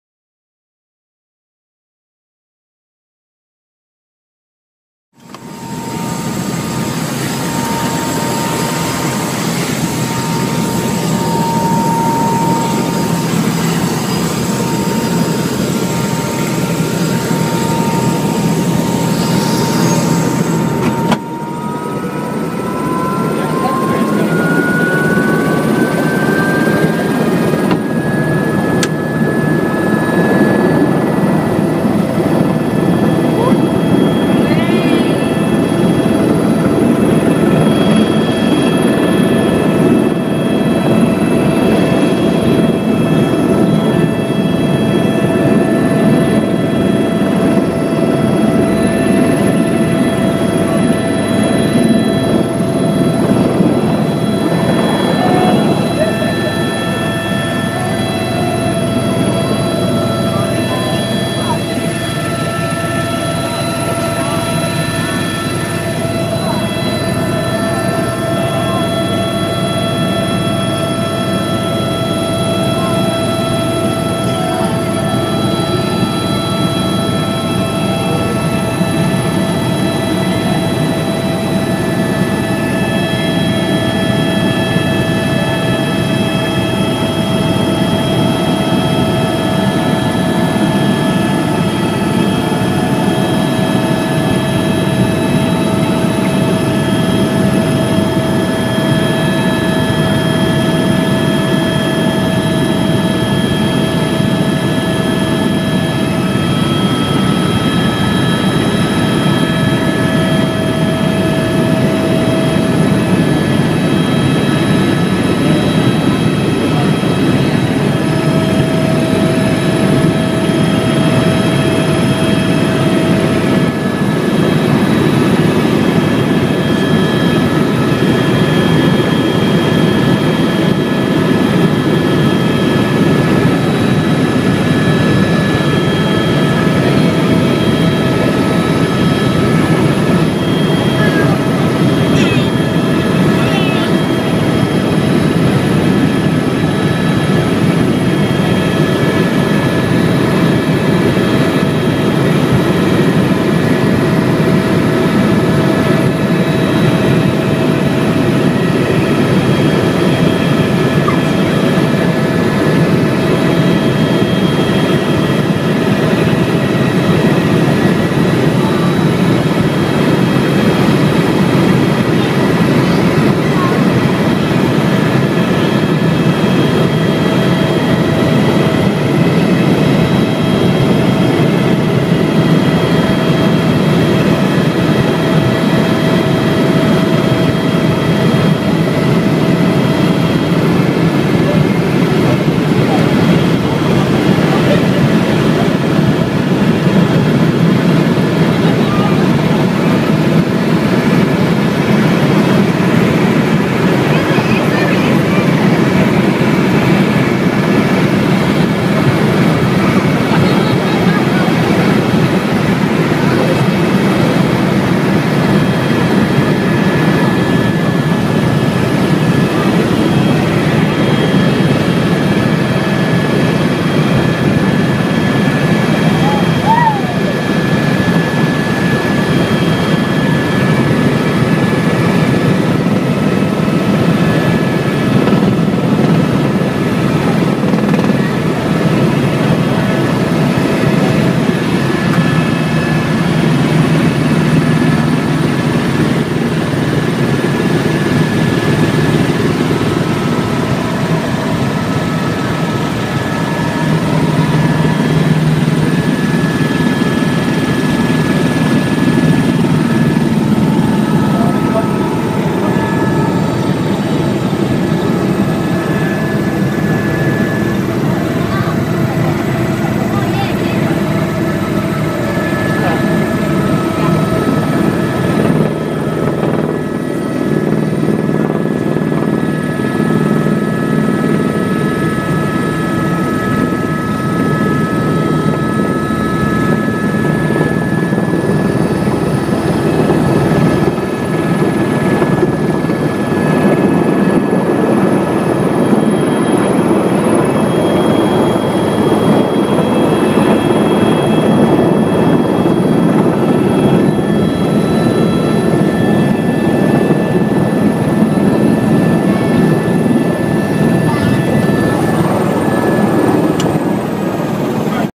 Silverstone GP Circuit Helicopter Tour 2013
A helicopter ride over silverstone, during the 2013 British GP weekend